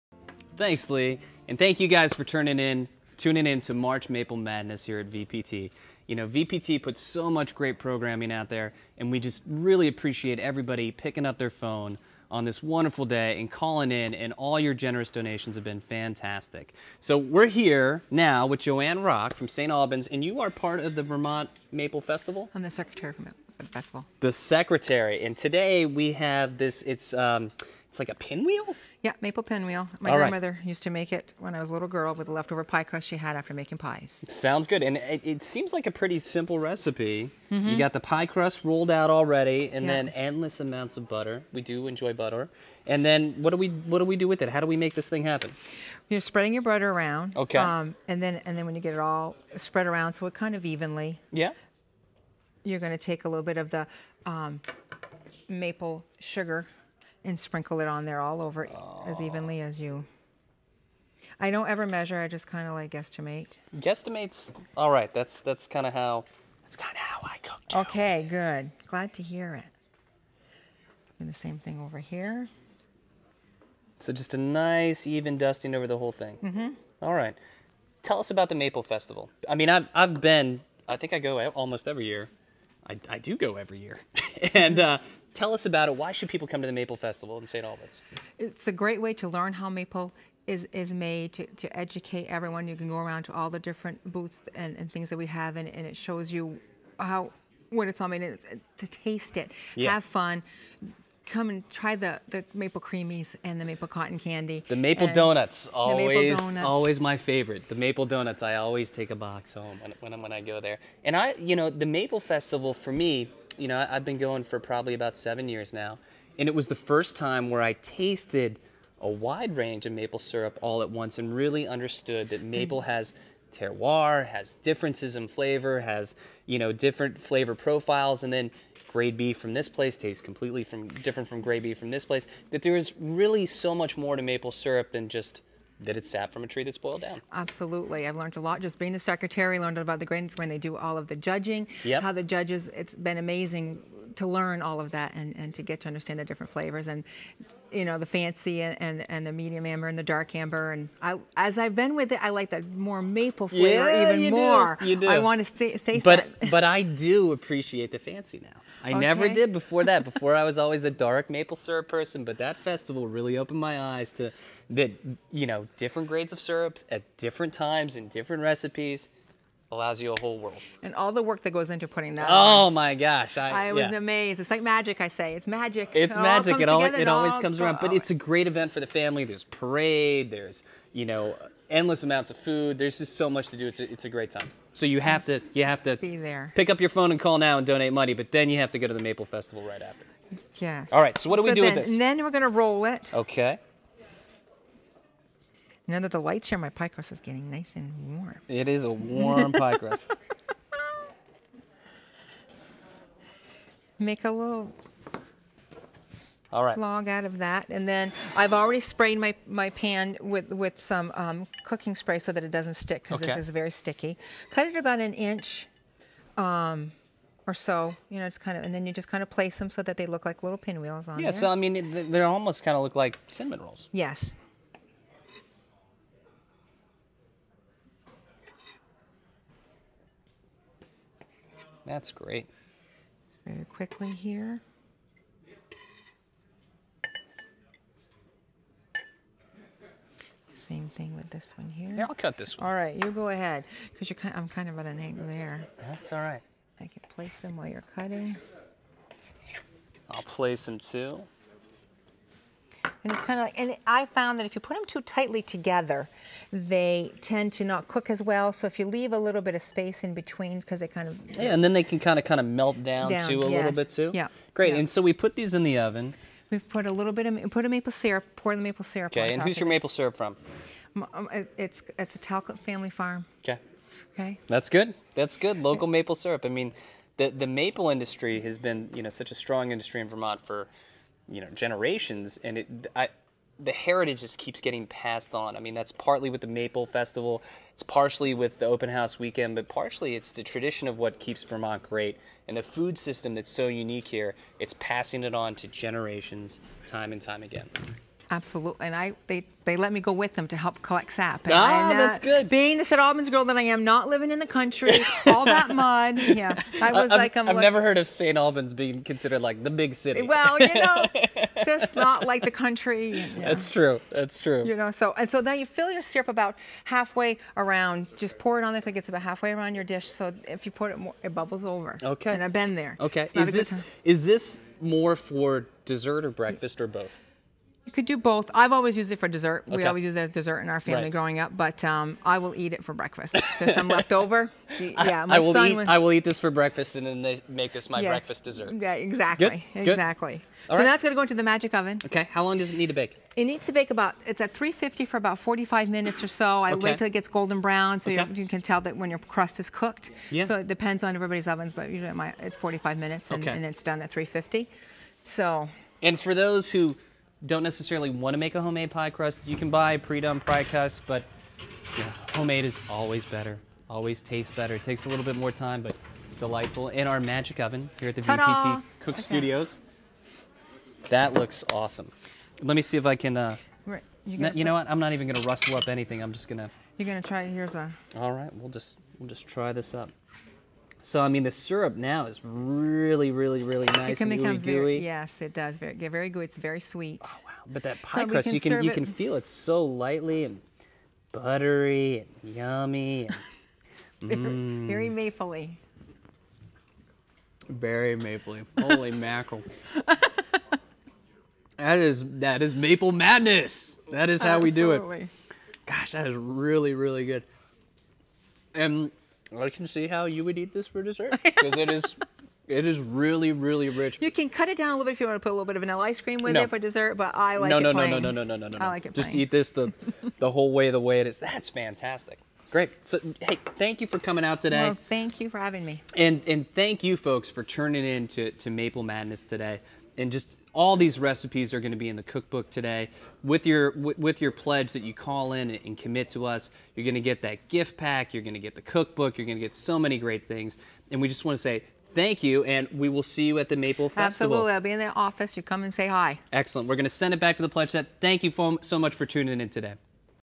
Maple Pinwheels
Recipe from Jo-Anne Rock, St. Albans, VT

1 ready made or homemade pie crust
2-4 Tablespoons of butter
1/2 - 3/4 cup of Maple Granulated sugar
3/4 - 1 cup of maple syrup (Medium or Dark Amber)

Roll out the pie crust on a cutting board.  Butter the surface of the pie crust.  Spread maple granulated sugar evenly over the buttered pie crust.  Roll it up into a log and cut into 1 inch pieces.  Place the pieces on their sides into an 8x8 baking pan.  Pour maple syrup so that it fills the pan half-way up the side of the pinwheels.  Caution: Do not over fill the pan with maple syrup as it will bubble over and make a very sticky mess.  Place baking pan on a cookie sheet and bake until golden brown.  (Approximately 45 to 60 minutes.)